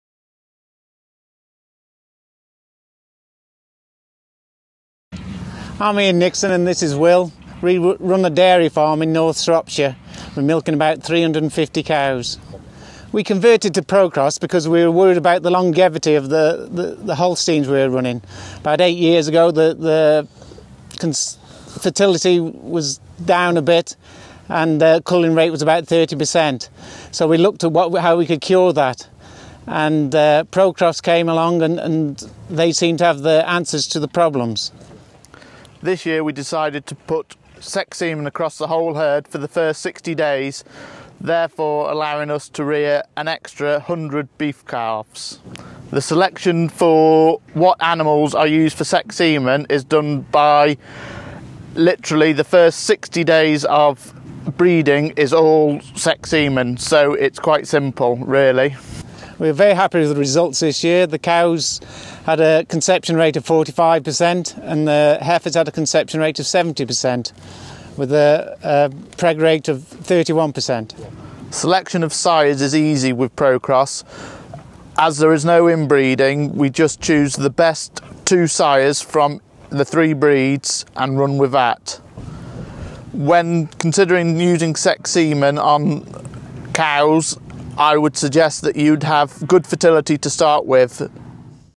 Ian and Will Nixon - ProCROSS de parto en bloque
El semen sexado consolida el éxito del hato ProCROSS de parto en bloque

Los márgenes se han disparado para un hato de cruzamiento ProCROSS que utiliza principalmente semen sexado X-Vik.

La reducción de los costos veterinarios y la venta de novillas excedentes otorgaron los resultados financieros.